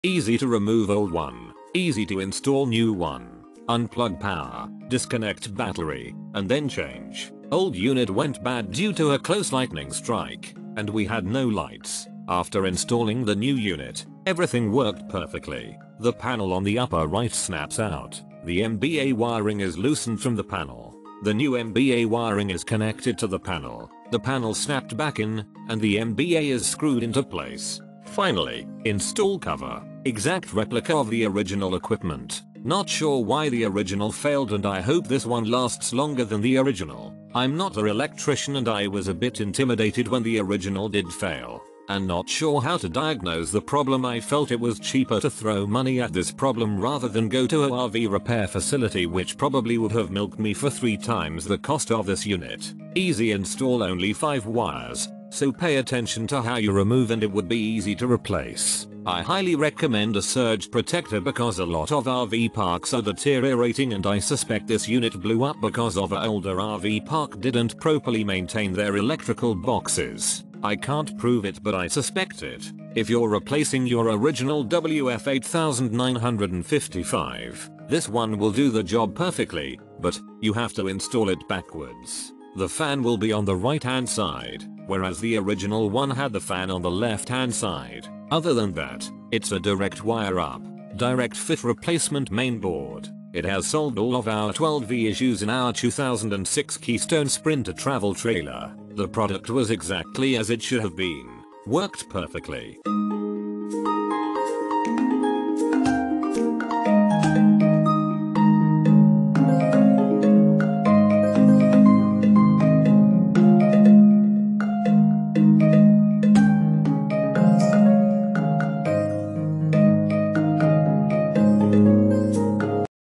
WFCO WF-8955 MBA Main Board Assembly - 55 Amp review
Main Features:
by entering your model number.. Automatic three-stage converter/charger. Power Input: 950W. Input Voltage: 105-130 VAC/60 Hz. Output Current: 55A. Output Voltage: 13.6 VDC (normal) / 13.2 VDC (trickle charge) / 14.4 VDC (bulk charge)